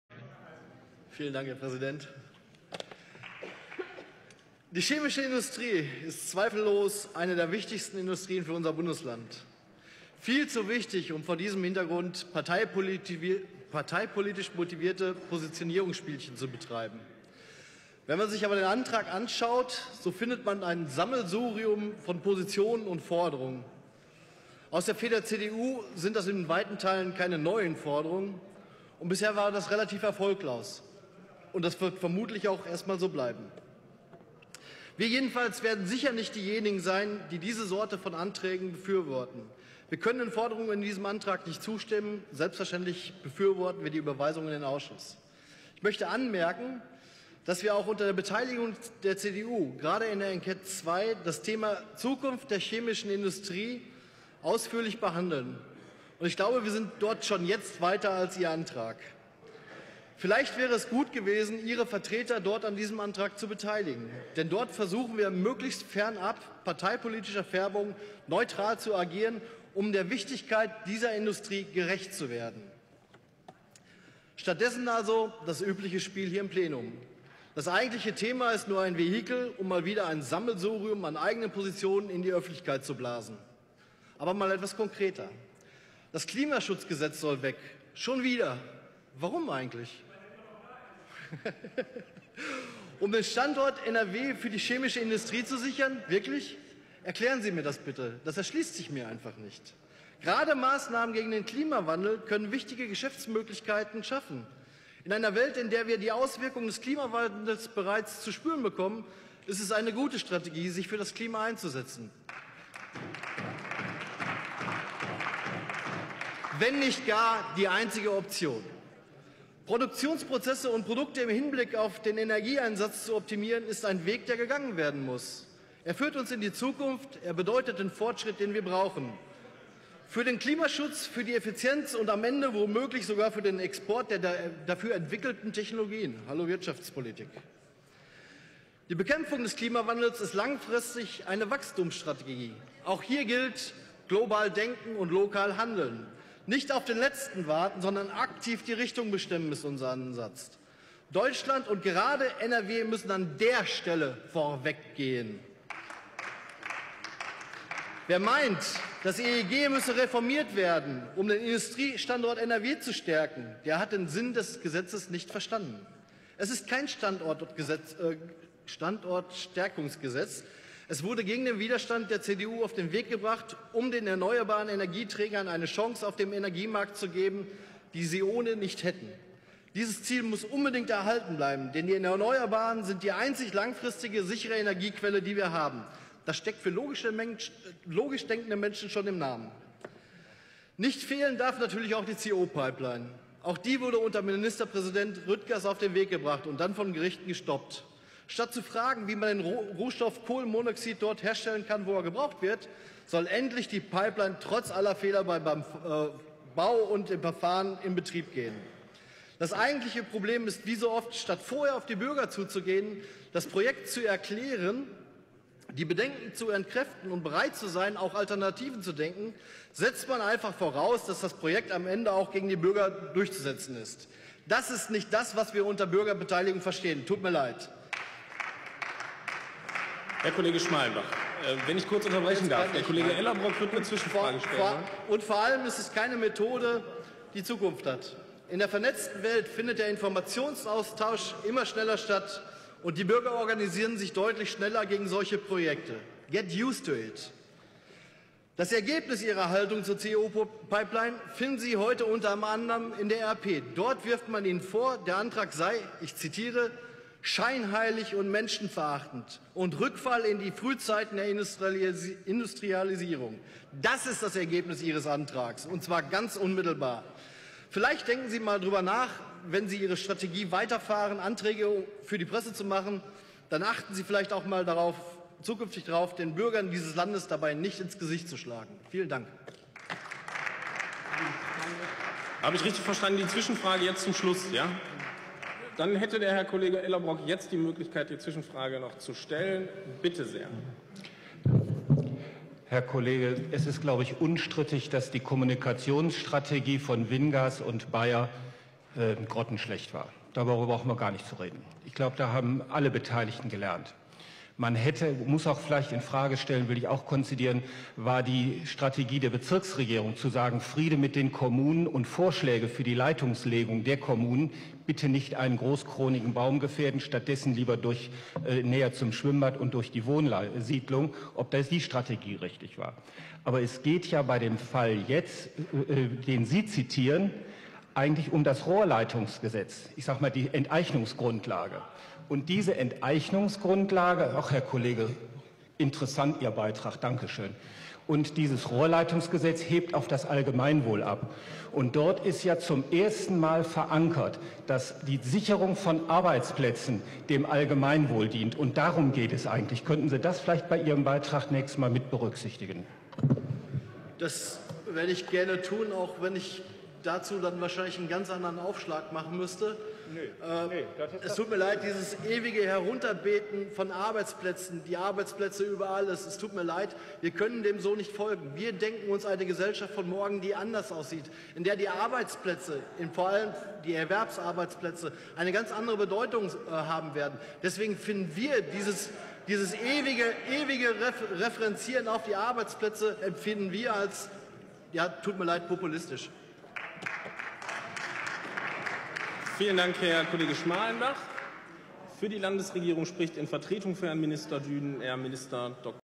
Kai Schmalenbach (@Dave_Kay) über die Wichtigkeit der chemischen Industrie in NRW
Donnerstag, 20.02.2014
Top 4. Chemische  Industrie muss Motor des Industriestandortes Nordrhein-Westfalen bleiben: Landesregierung muss heute Grundlagen für Wachstum von morgen schaffen
Drucksache 16/5040
Abstimmungsempfehlung: Zustimmung zur Ausschussüberweisung